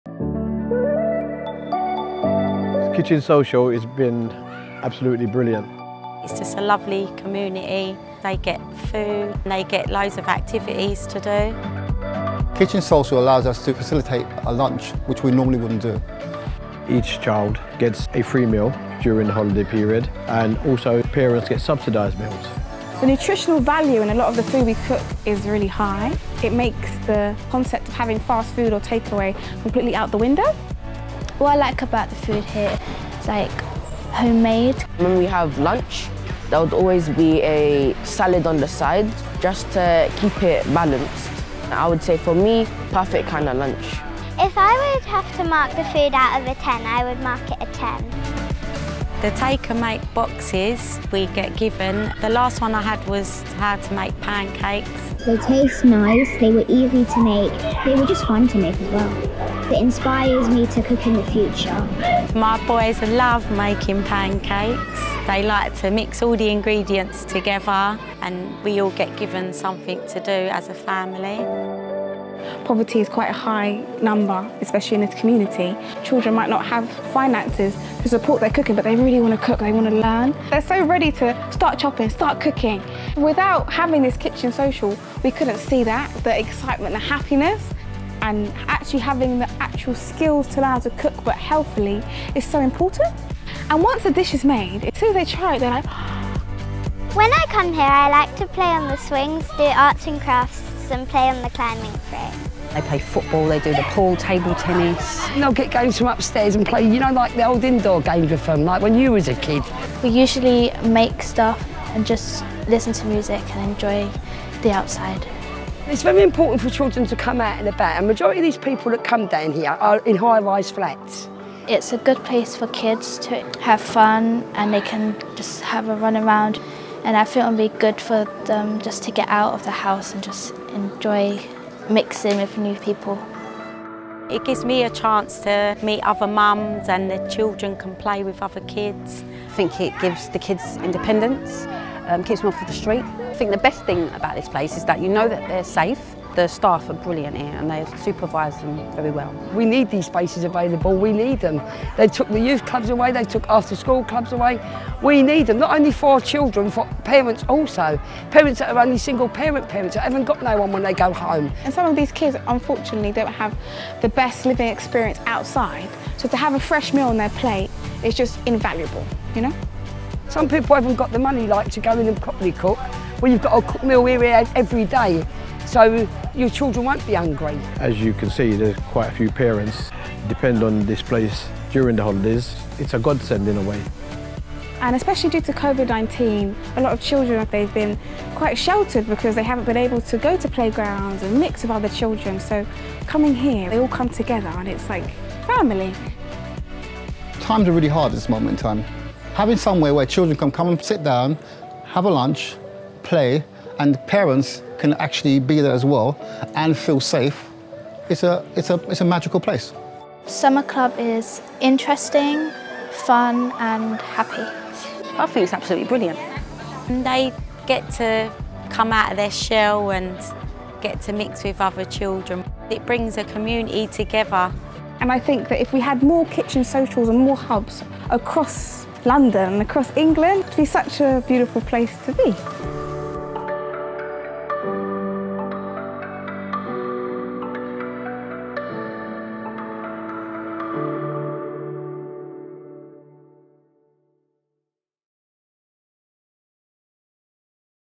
What is Kitchen Social? | Mayor's Fund for London
There are estimated to be at least 500,000 children living in households experiencing food insecurity as a result of Covid-19. Holiday food and activity programmes like Kitchen Social are vital in helping to reach and support the hardest-hit families.

Kitchen Social, a Mayor’s Fund for London programme, is the largest independent provider of food for children in London during the school holidays.

Kitchen Social is a programme that addresses the growing and serious issue of children and young people facing food insecurity and social isolation outside of term time. Poor nutrition and isolation not only affect a child’s physical wellbeing but also their long-term educational attainment and employment prospects, thereby perpetuating the cycle of poverty.

The programme gives children and young people a safe place to go during the school holidays where they can socialise, make new friends and get a free, healthy meal.

Over the 2020/21 academic year, the programme supported 22,428 children and young people and provided 85,988 meals.

For more information about the Mayor's Fund for London and the Kitchen Social programme, please visit our